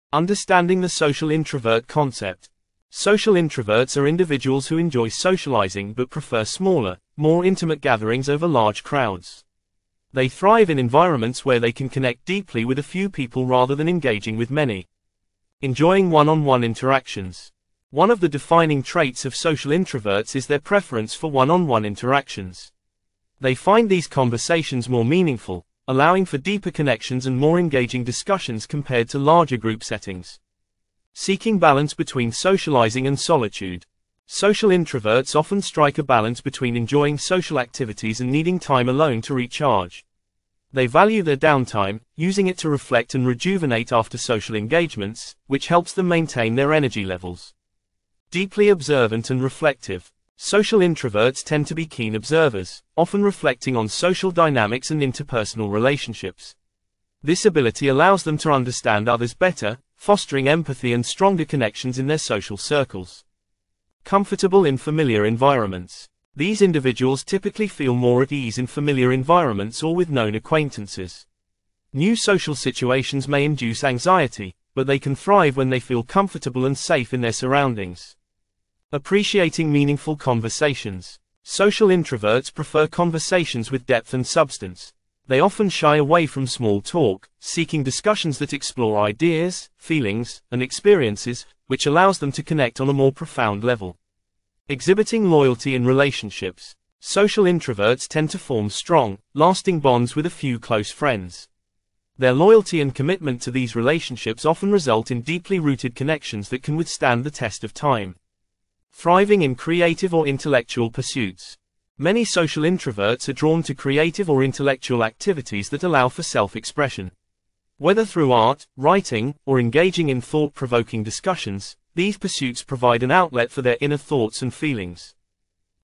What Are the Key Traits of a Social Introvert
What makes a social introvert? Explore the unique traits that define social introverts and how they navigate social situations.